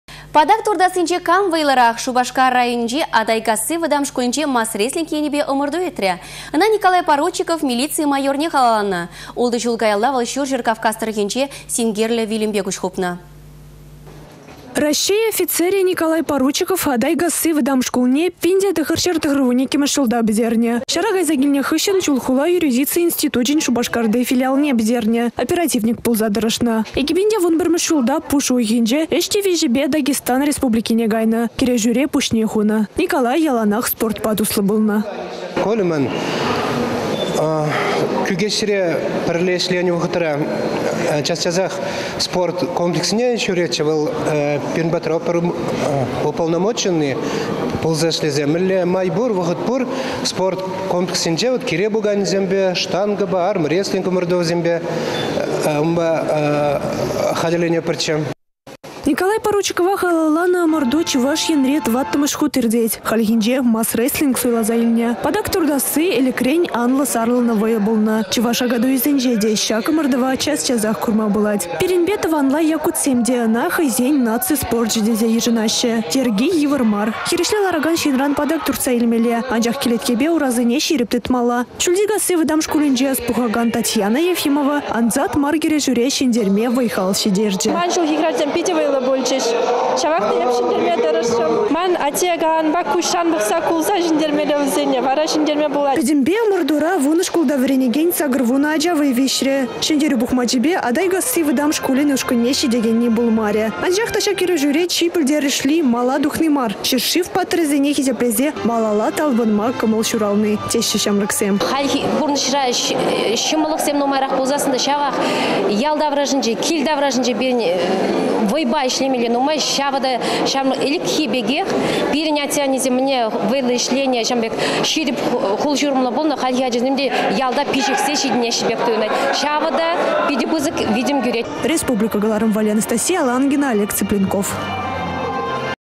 Николай Поручикова халалланă ăмăрту
Патак туртассинче кам вăйлăрах? Шупашкар районĕнчи Атайкасси вăтам шкулĕнче мас-рестлинг енĕпе ăмăрту иртрĕ. Ăна Николай Поручиков милици майорне халалланă. 6 çул каялла вăл Çурçĕр Кавказ тăрăхĕнче синкерте вилнĕ.